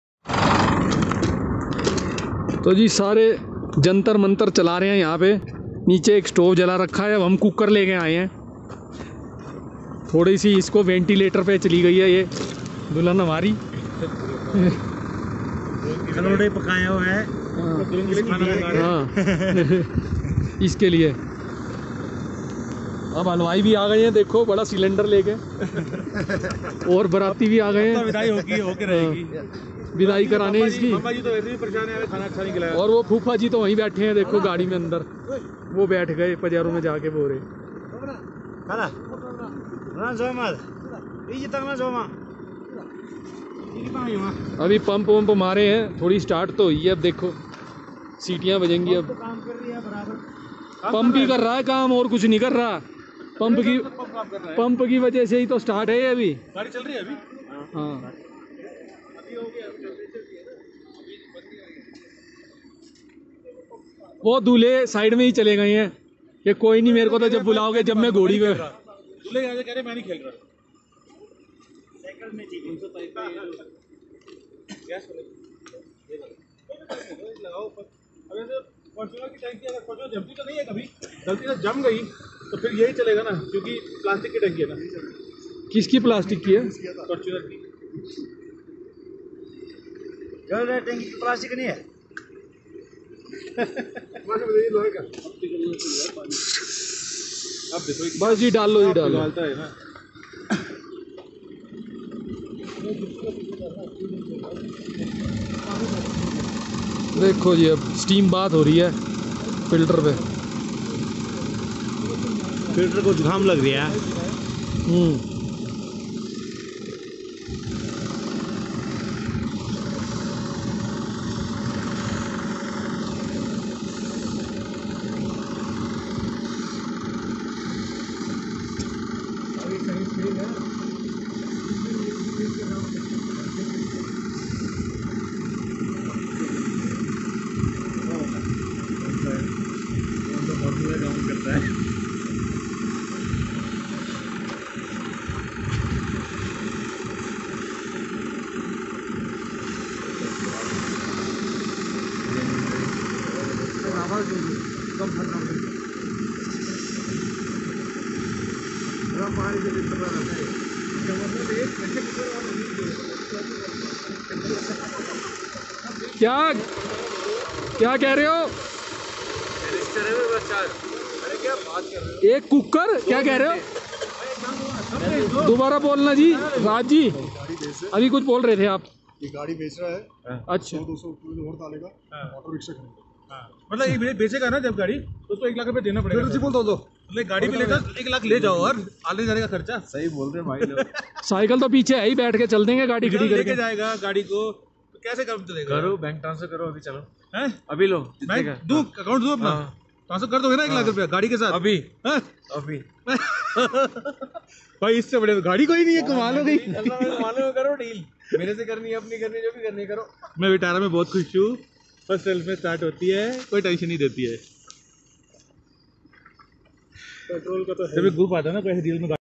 How we started ISUZU in Spiti Valley Sub Zero Diesel Froze with cooker
This is when we used cooker to start ISUZU Dmax. You can also follow same technique if stuck in winter diesel freezing conditions.